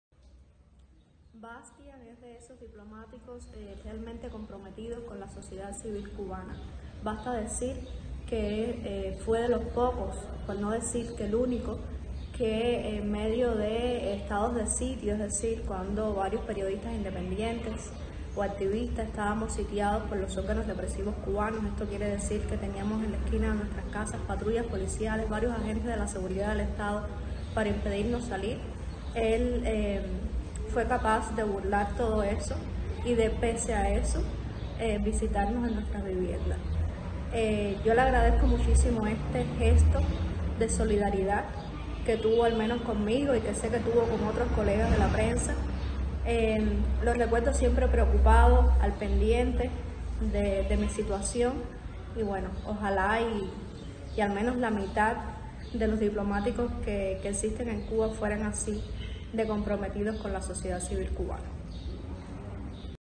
Referentes democráticos de Cuba saludan al diplomático holandés Bastiaan Engelhard
Mensaje de Camila Acosta a Bastiaan Engelhard